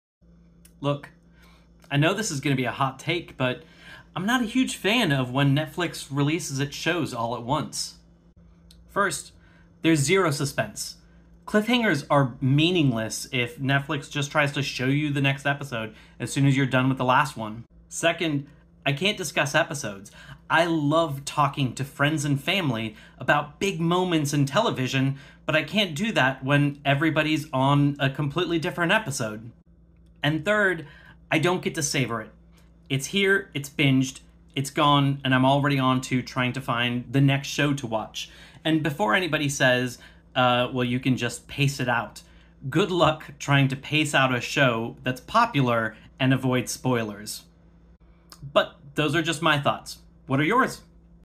I don't like Netflix's business model
I am not a fan of binging great television. I think that while Netflix's model provides instant gratification and can keep people on the platform for hours at a time after a show is dropped, it is a worse experience for viewers in the long run.